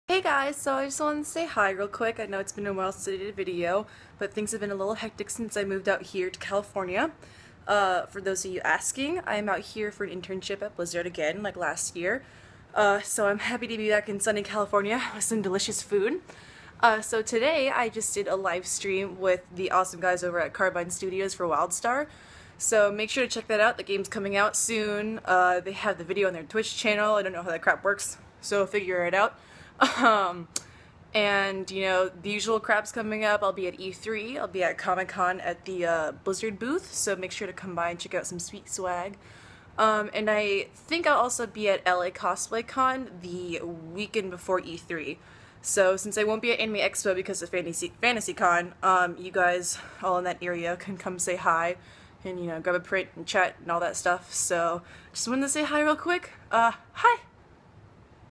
HI! Updates and other crap!
So I literally just took this video 2 seconds ago and didn't even go over it to see if it looked bad. No editing or anything I'M SORRY.

Just wanted to say hi and update you guys! Next cons: E3 & SDCC for work and then LA Cosplay Con for cosplay!